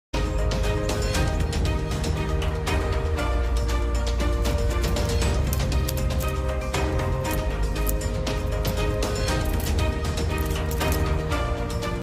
Super aufgeregtes Eichhörnchen |Squirrel is extremely excited 😍
Eichhörnchen ist sehr aufgeregt wegen der Nüsse